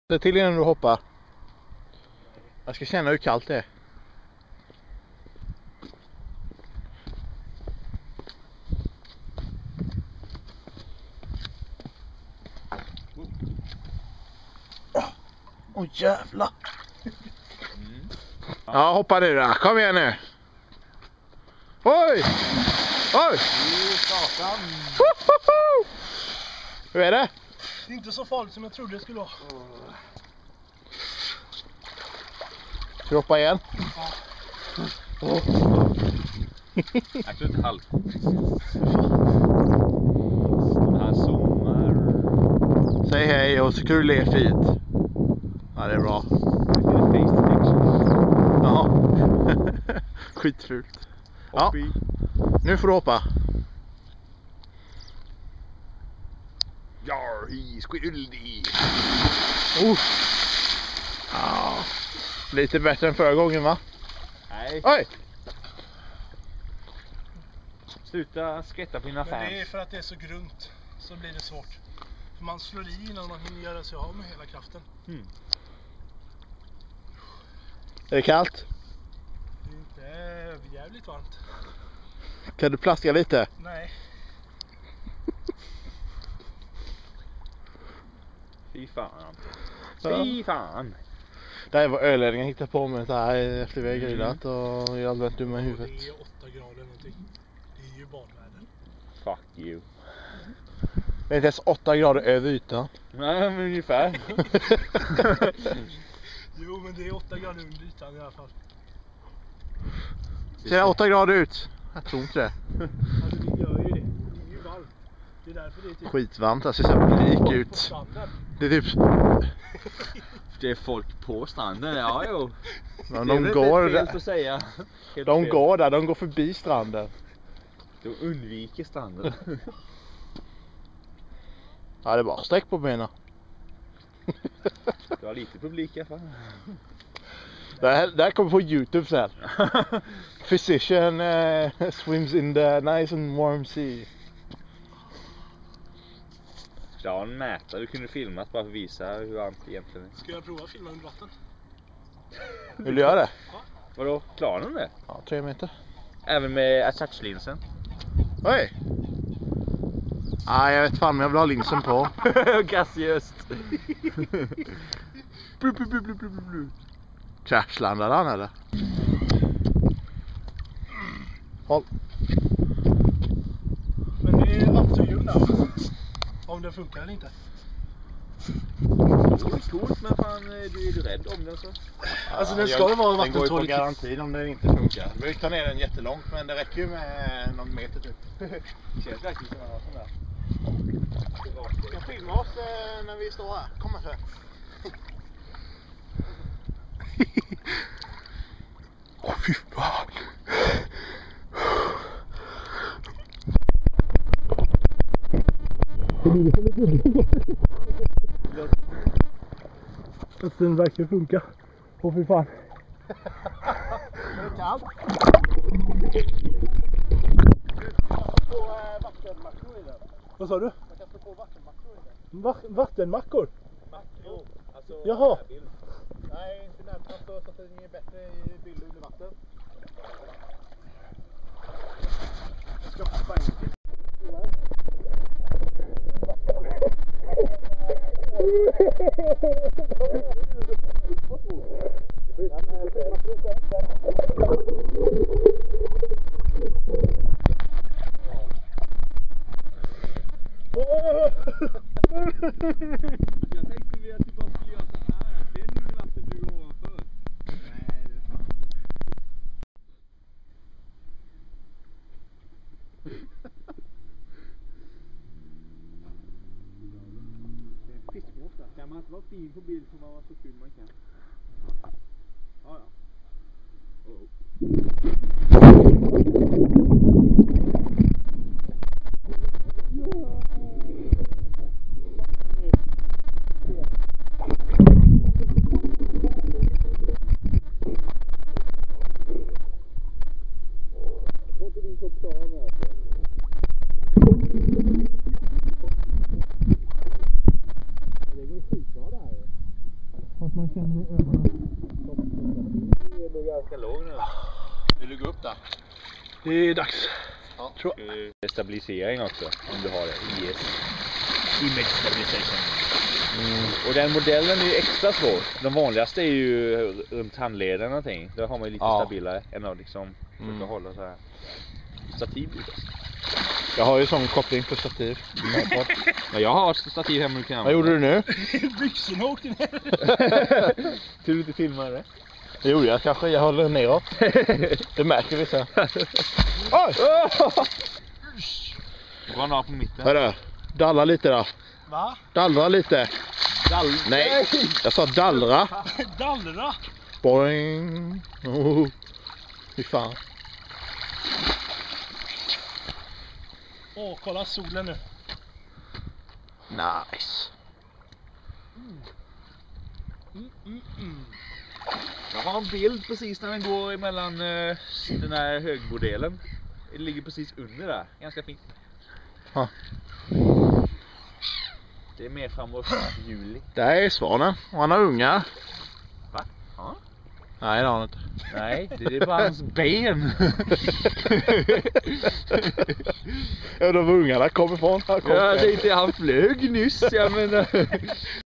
Första doppet för året April 2011
Första doppet i år under påsken April 2011,
vi passade på att testa om playsport kameran funkade under vatten också :-).